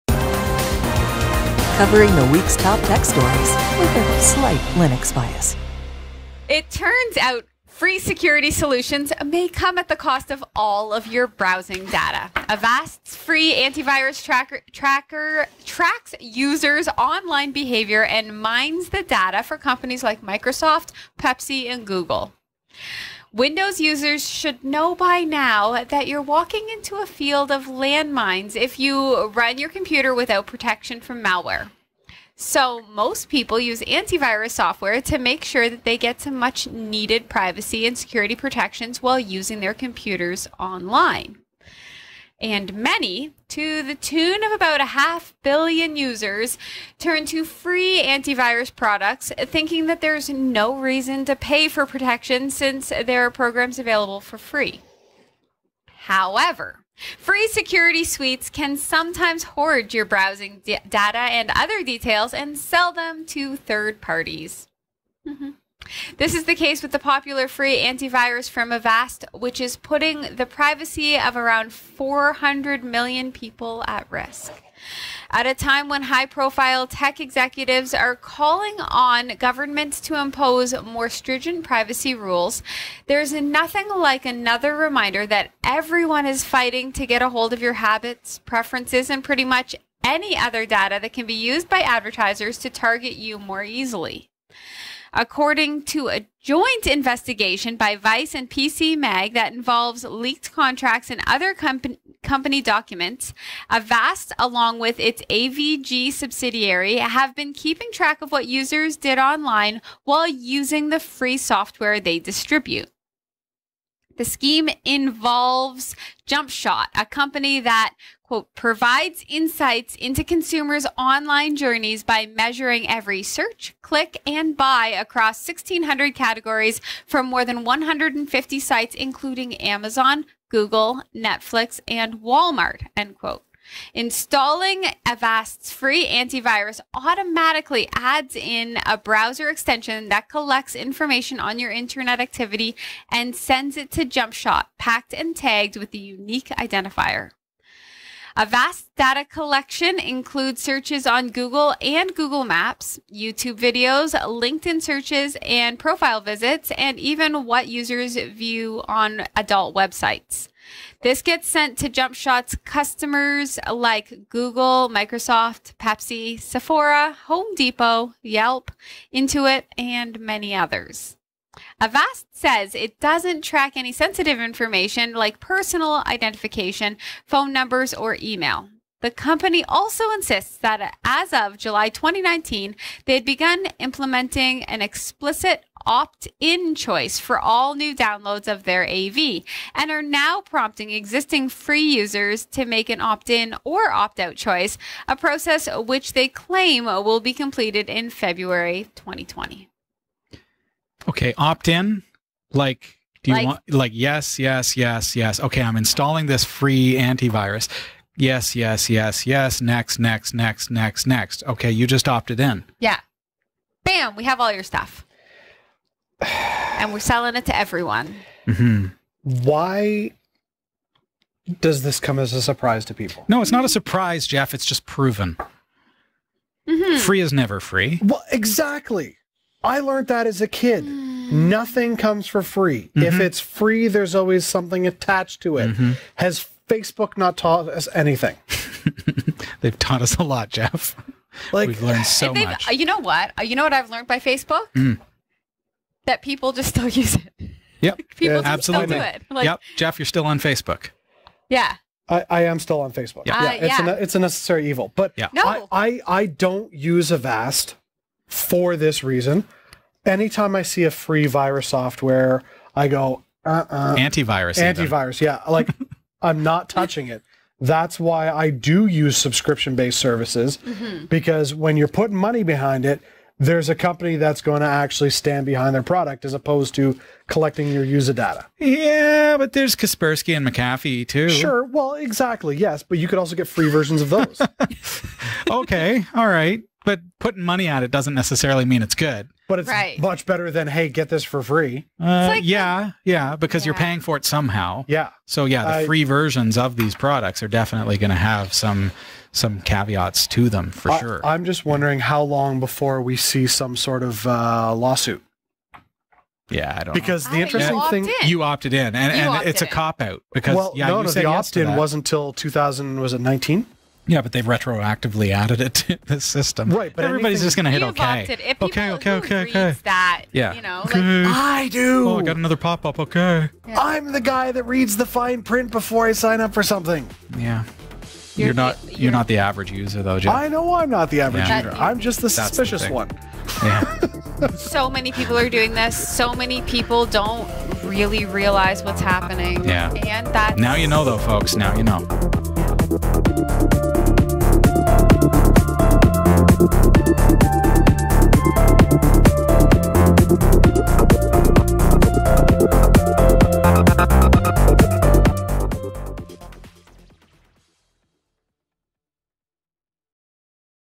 Surprise: Your Free Antivirus is Tracking and Selling Your Private Data
It turns out free security solutions may come at the cost of all your browsing data: Avast's free antivirus tracks users' online behavior and mines the data for companies like Microsoft, Pepsi, and Google.